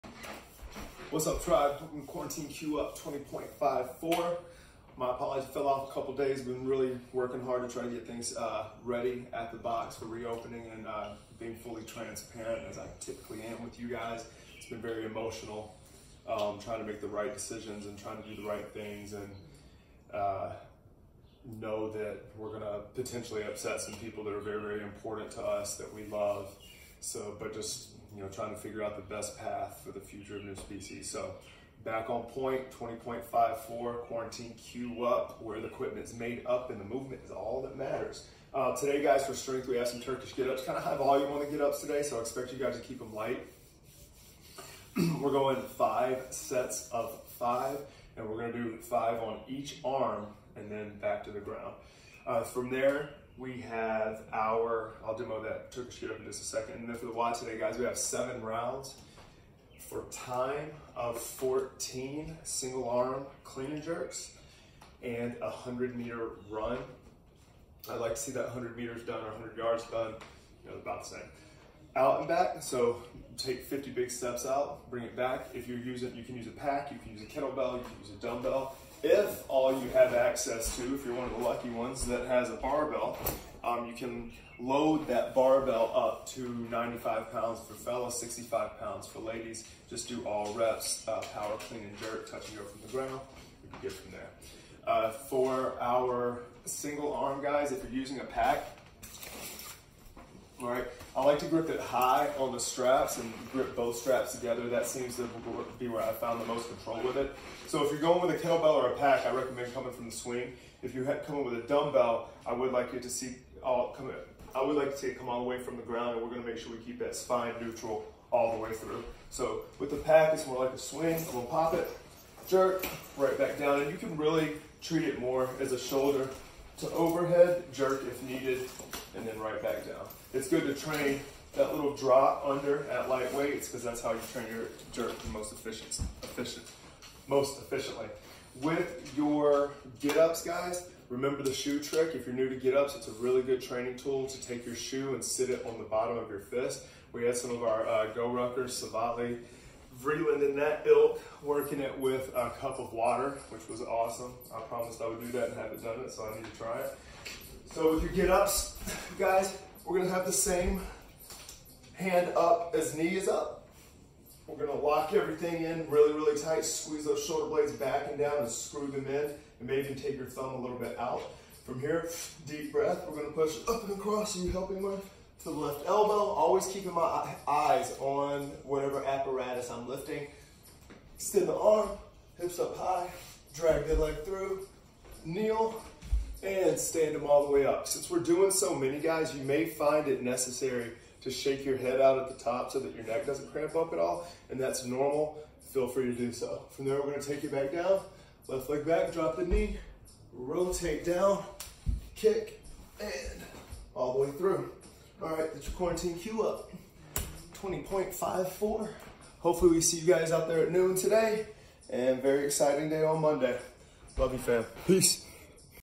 Quarantine Cue Up 20.54
Strength:Turkish getup, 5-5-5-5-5, (each side)WOD:7 rounds for time:100m run14 Single arm clean and jerks, ~50/35lbsPost loads and times to beyond the whiteboard.